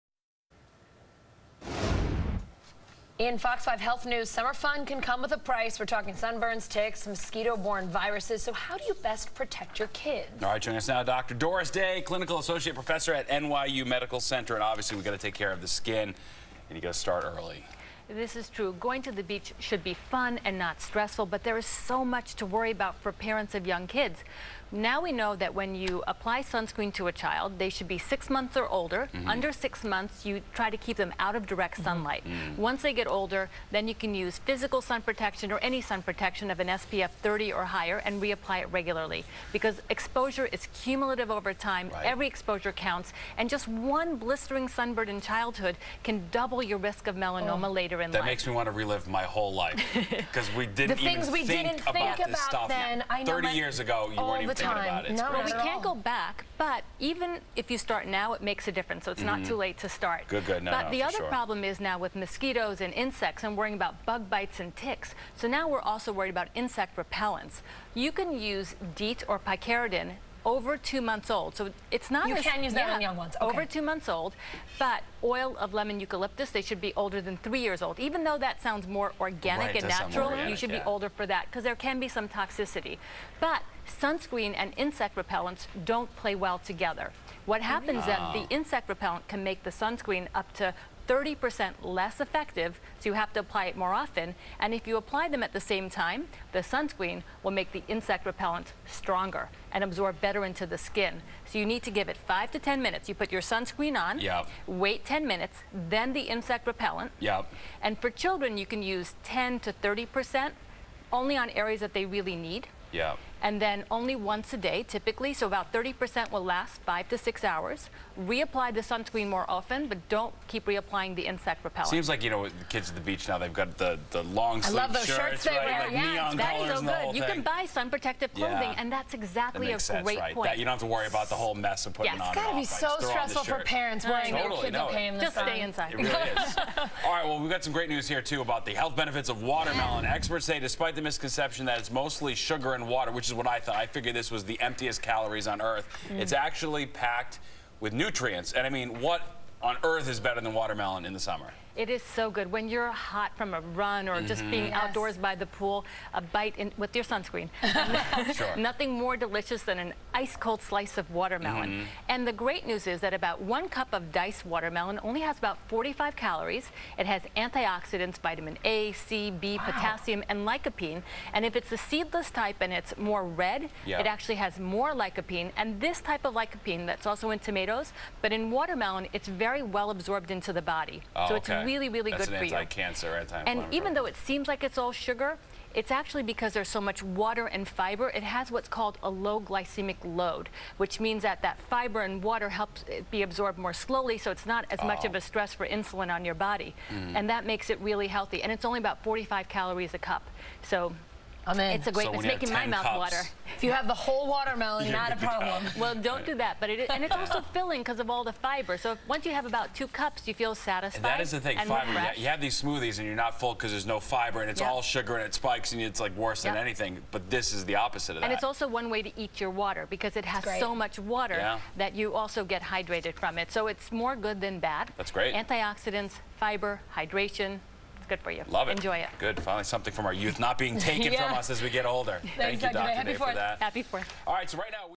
Warning: Sunscreen and Bug Spray may be a dangerous mix
Dr Doris Day tells you what you need to know about using sunscreen and bug spray safely this summer season.
Also, why watermelon may be a great way to stay hydrated in the hot summer months.